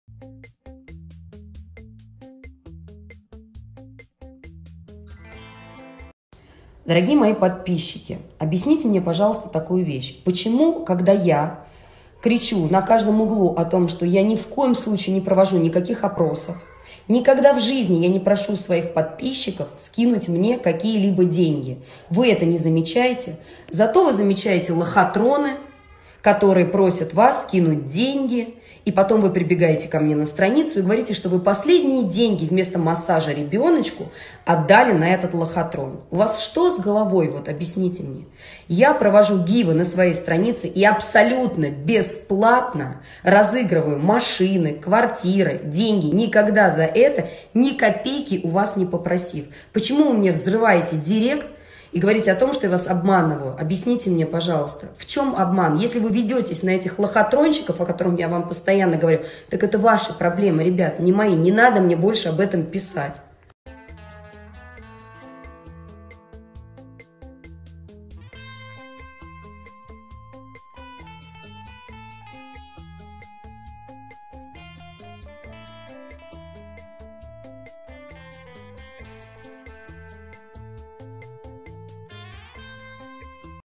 Мошенники в опросах! Обращение Бородиной (ondom2.com)
В этом выпуске новости и слухи  про Ксению Бородину, которая сообщила о мошенниках в опросах.